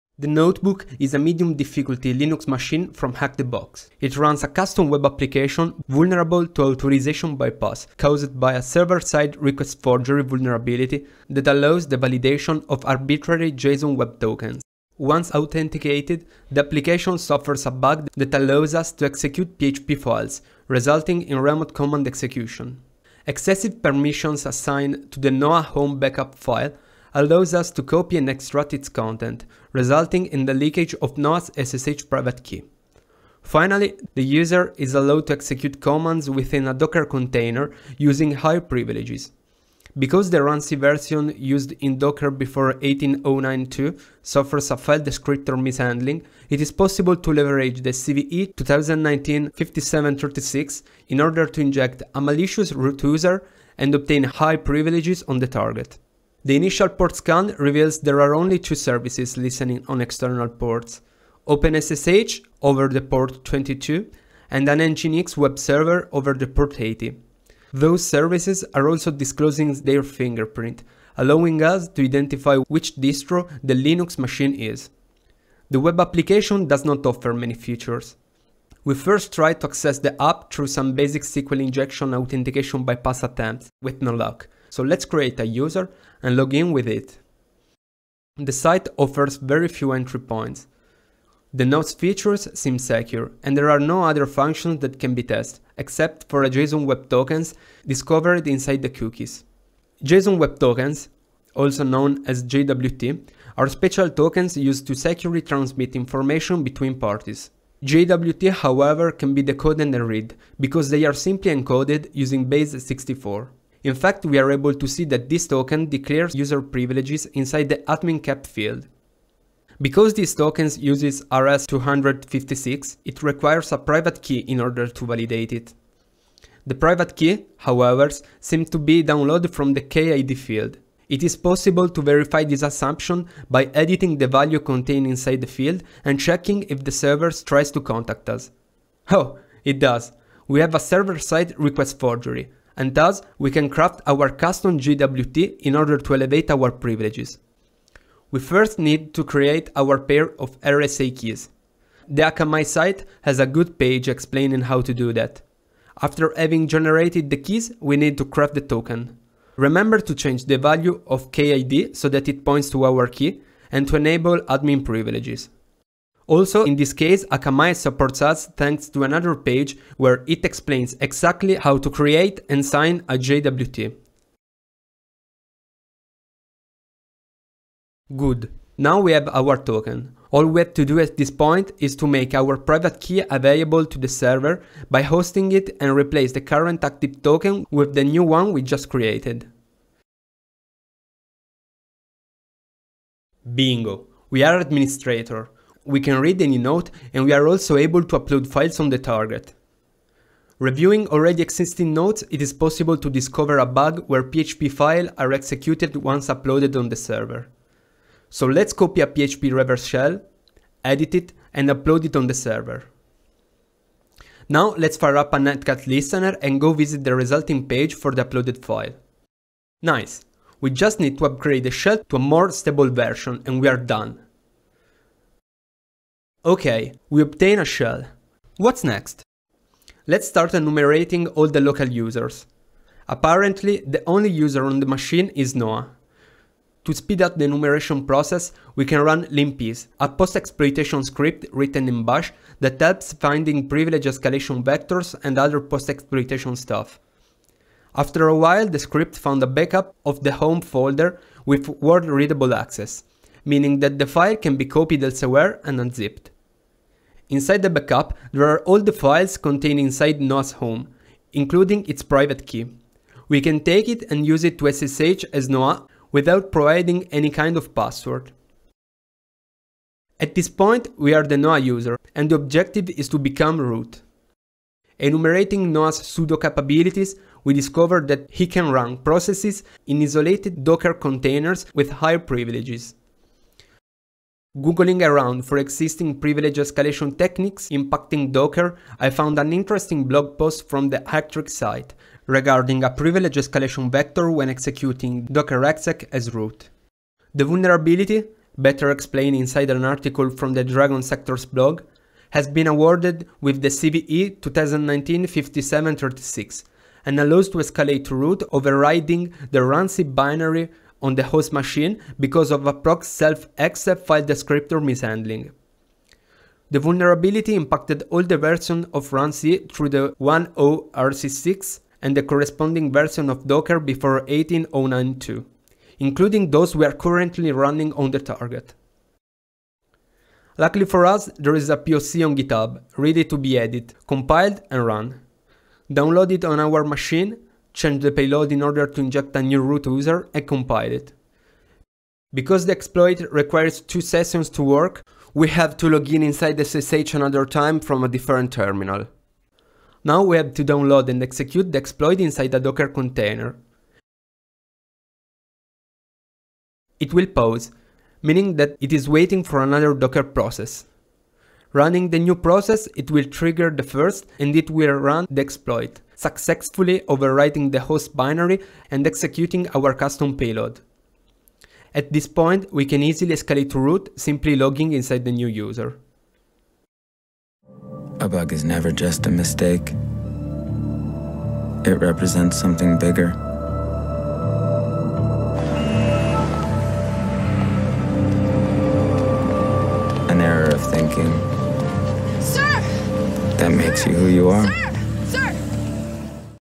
SSRF, arbitrary JWT validation & runc privilege escalation | TheNotebook @ HackTheBox
TheNotebook is a medium difficulty Linux machine from @HackTheBox. It runs a custom web application vulnerable to authorization bypass caused by a Server Side Request Forgery vulnerability that allows the validation of arbitrary Json Web Tokens. Once authenticated, the application suffers a bug that allows us to execute PHP files, resulting in Remote Command Execution. Excessive permissions assigned to the Noah home backup file allows us to copy and extract its content, resulting in the leakage of Noah's SSH private key. Finally, the user is allowed to execute commands within a docker container using high privileges. Because the runc version used in Docker before 18.09.2 suffers a file-descriptor mishandling, it is possible to leverage the CVE-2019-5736 in order to inject a malicious root user and obtain high privileges on the target.

=== Timestamp ===
00:00 Intro
00:56 Target enumeration
01:59 Exploiting SSRF to arbitrary validate an administrator self-signed JWT
03:20 Exploiting a bug that allows to execute custom PHP code
03:49 Discovering a backup file containing local user's SSH private key
04:34 Privilege escalation exploiting runc (CVE-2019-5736)
06:17 Conclusions